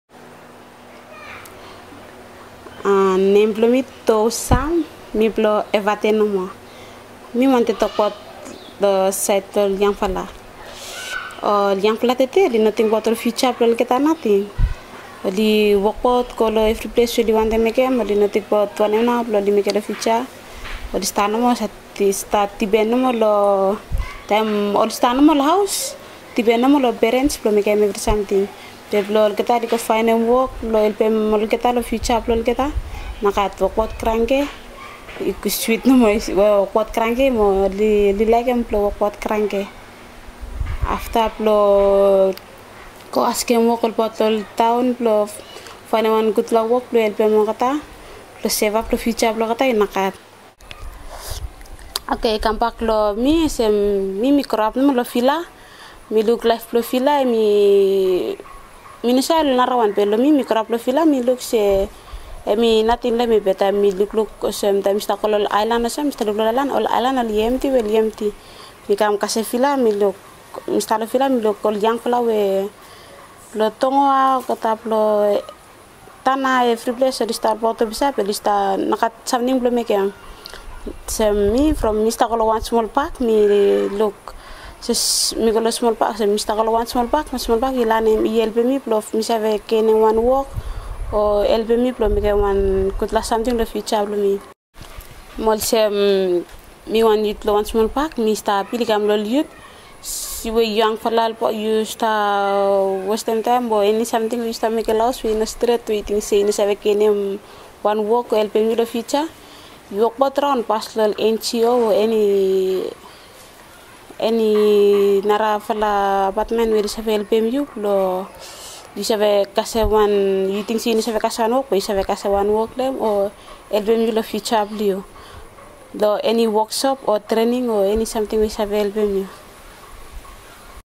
Toukala speaking Bislama | Pacific Islander languages | Wikitongues
Toukala speaks Bislama, an English Creole language and lingua franca for diverse Pacific Islander communities in Vanuatu, an archipelago nation in the South Pacific region of Melanesia. This video was recorded by Sarah Doyle in Port Vila, Vanuatu.